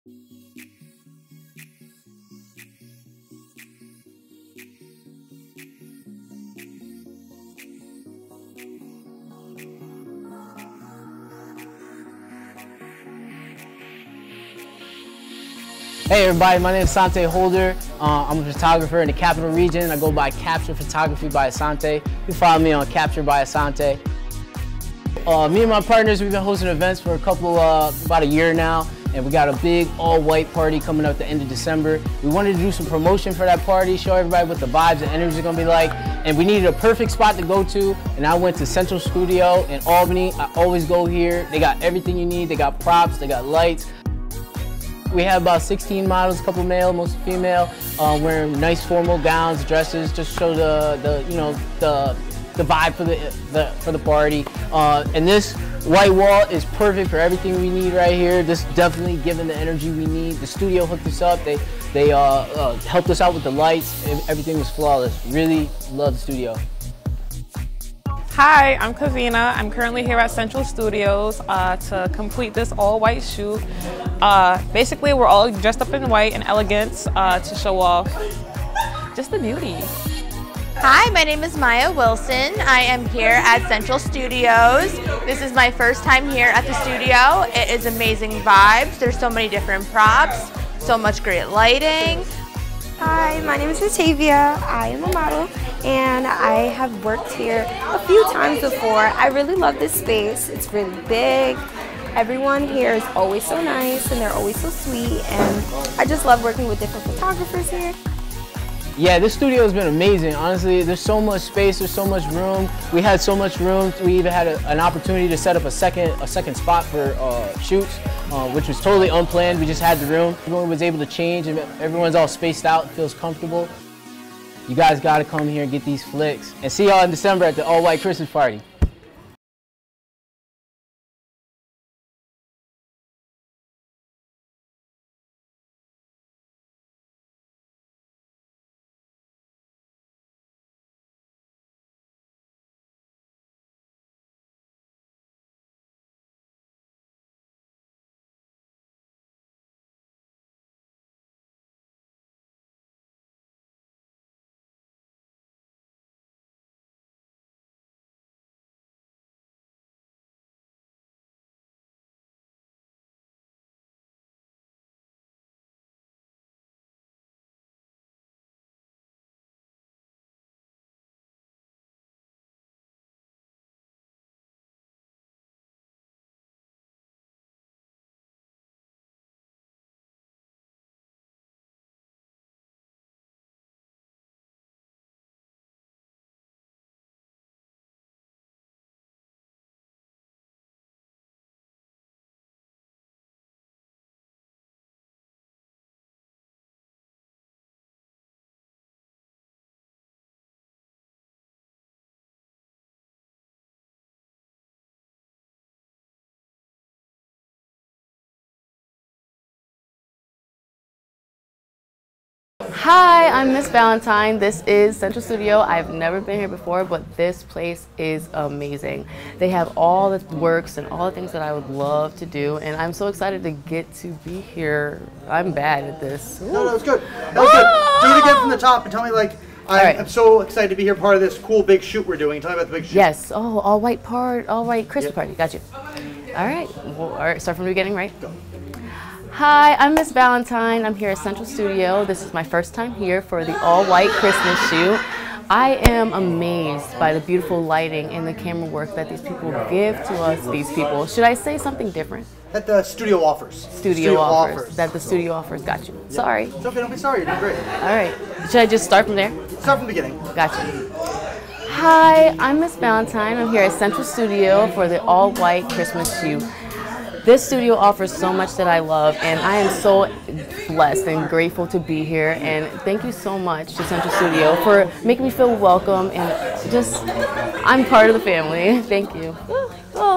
Central Studio - Behind the Shoot - All White Party
Quick Look Behind the Shoot of Captures by Asante's Latest Project in Central Studio, the ALL White Party. Central Studio is a rental studio in Albany NY.